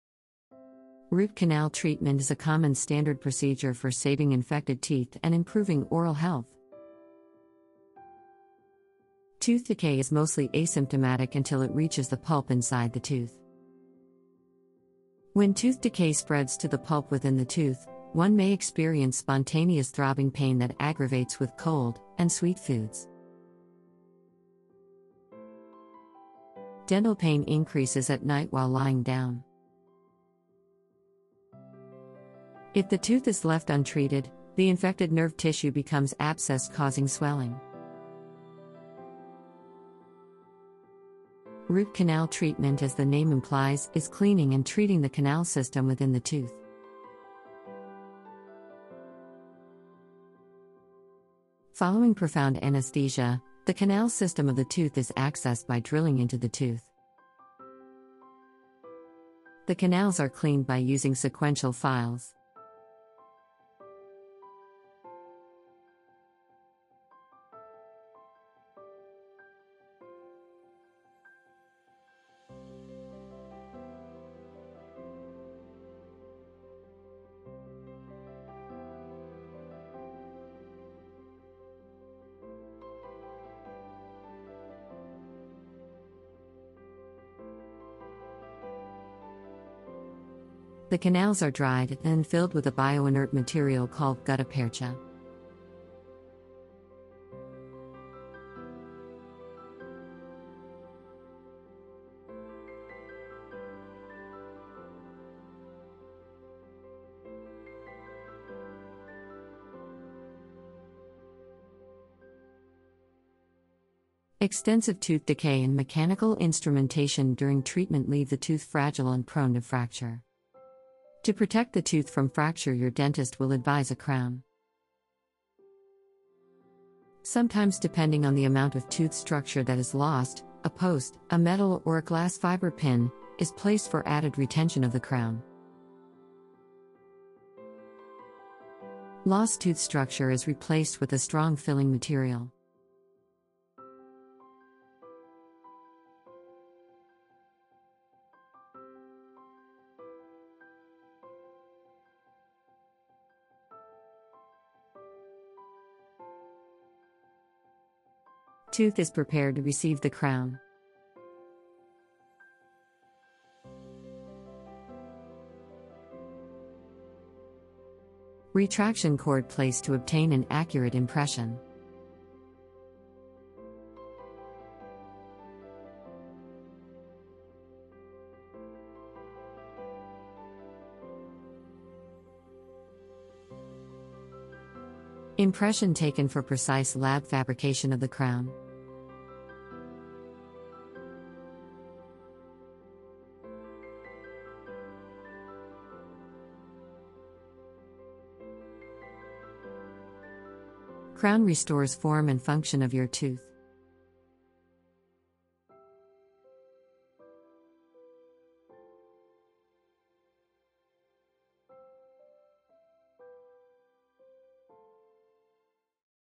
Root Canal Treatment Explained Step-by-Step | No Pain, Just Relief!
Root Canal? Root Canal Treatment is a common, standard procedure for saving infected teeth and improving oral health. The dentists remove the infected pulp and fill the canal of the tooth with suitable dental materials.

1 . Tooth decay is mostly asymptomatic until it reaches the pulp (nerve tissue) inside the tooth.
2. When tooth decay spreads to the pulp (nerve tissue) within the tooth, One may experience spontaneous throbbing pain that aggravates with cold and sweet foods.
3. Dental pain increases at night while lying down.
4. If the tooth is left untreated, the infected nerve tissue becomes abscessed causing swelling.
5. Root canal treatment as the name implies is the cleaning and treating the canal system within the tooth.
6. Following profound anesthesia, the canal system of the tooth is accessed by drilling into the tooth.
7. The canals are cleaned by using sequential files.
8. The canals are dried and then filled with a bio-inert material called gutta percha.
9. Extensive tooth decay and mechanical instrumentation during treatment leaves the tooth fragile and prone to fracture.
10. To protect the tooth from fracture your dentist will advise a crown.
11. Sometimes depending on the amount of tooth structure that is lost, a post (a metal or a glass fiber pin) is placed for added retention of the crown.
12. Lost tooth structure is replaced with a strong filling material.
13. Tooth is prepared to receive the crown.
14. Retraction cord placed to obtain an accurate impression.
15. Impression is taken for precise lab fabrication of the crown.
16. Crown restores form and function of your tooth.